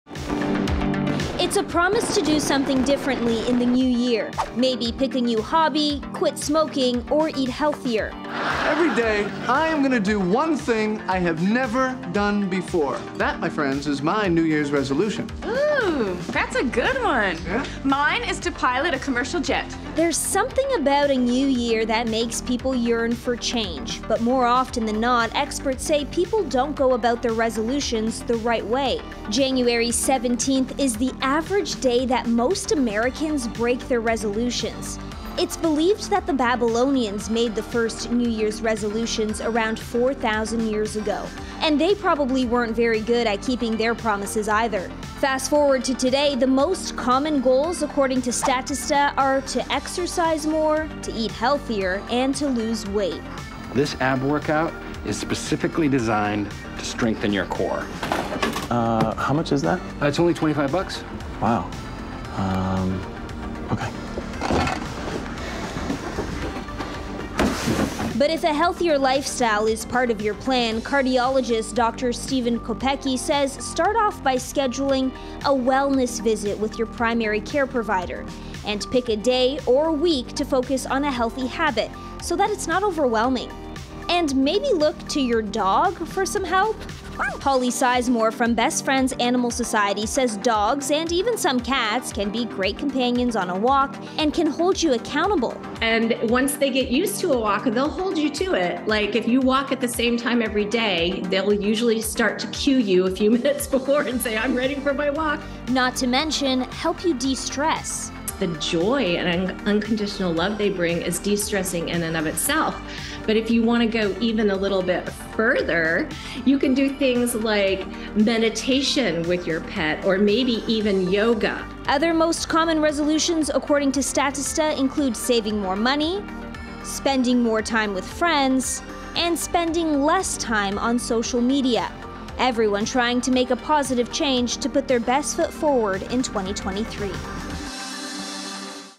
How to make sure you keep your New Year's resolutions in 2023
New year, new me... we've heard this saying time and time again, but few people actually keep their new years resolutions. As many are feeling burnt out from the holidays - and looking to make big life changes in 2023 - experts say focus on small steps and meaningful habits.